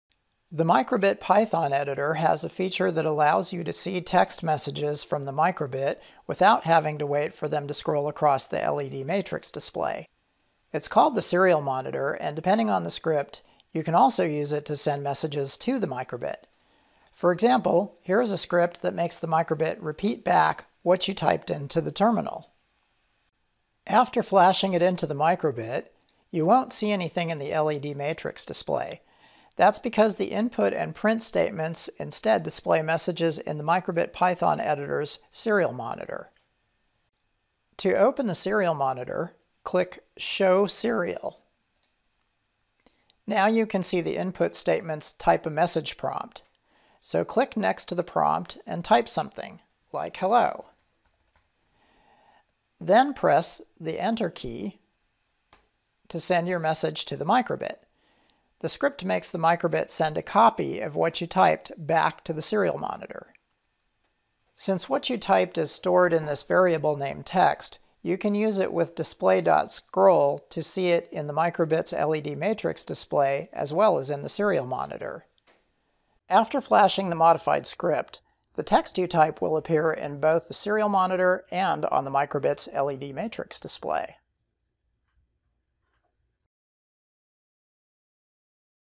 Use micro:bit Python Editor V3 Serial Monitor
Step-by-step instructions for how to use the micro:bit Python Editor serial monitor.